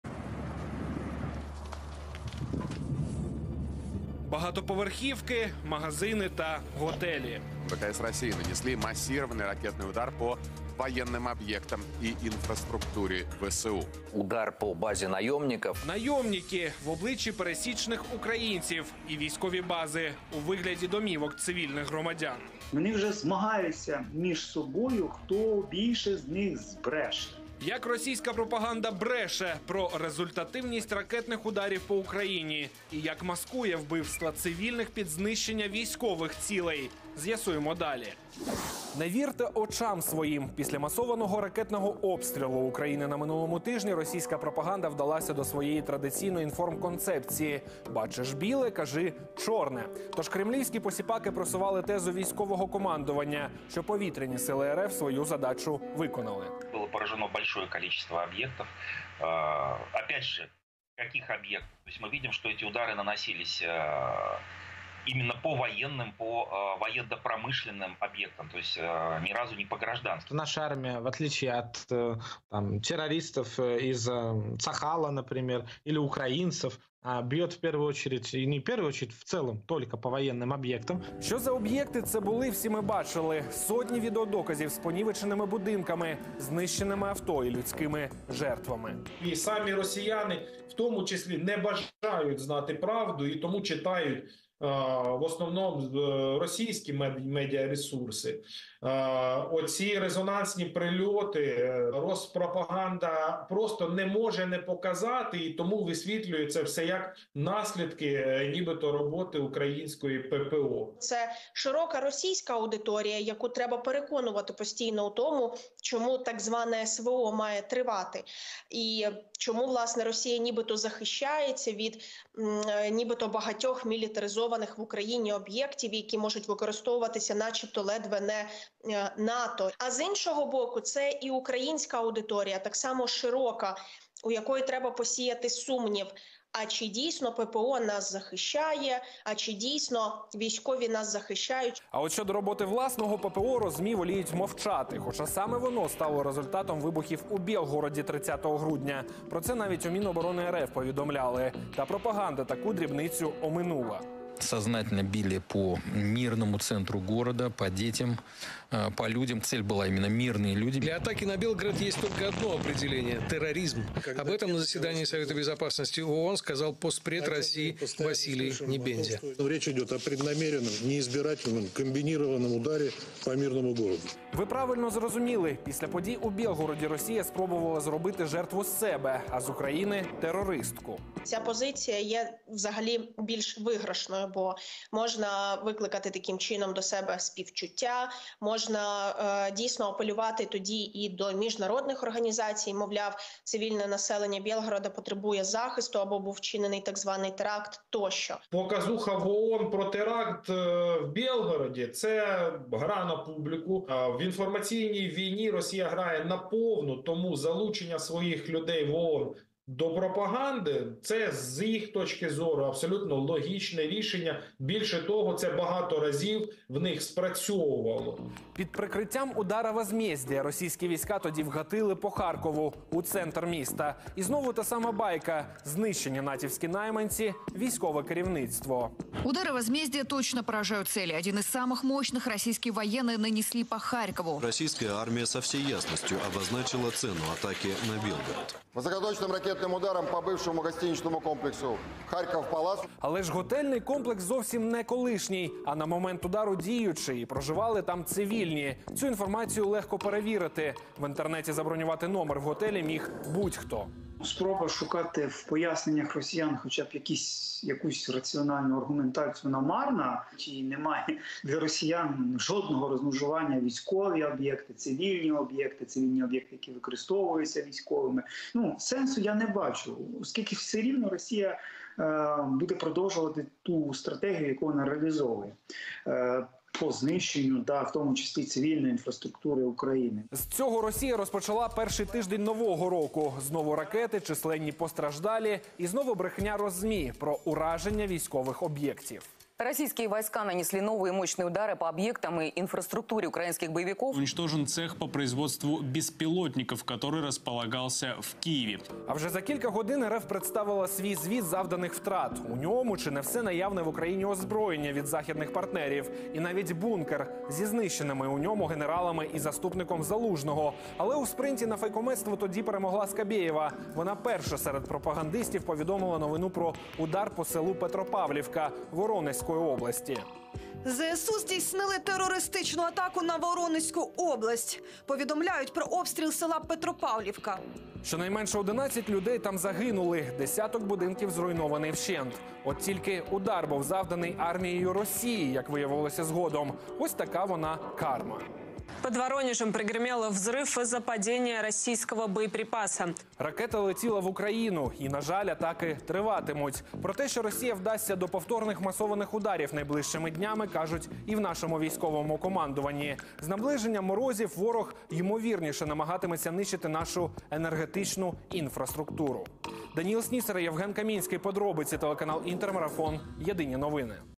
росія - жертва, а Україна - терористка: як пропаганда висвітлювала ракетні атаки по Україні
Прикриваючись бавовною в Бєлгороді, росЗМІ намагалися довести власним глядачам і міжнародній спільноті, що масовані ракетні атаки на Україну - виправдані. А всі цілі - це воєнні обʼєкти.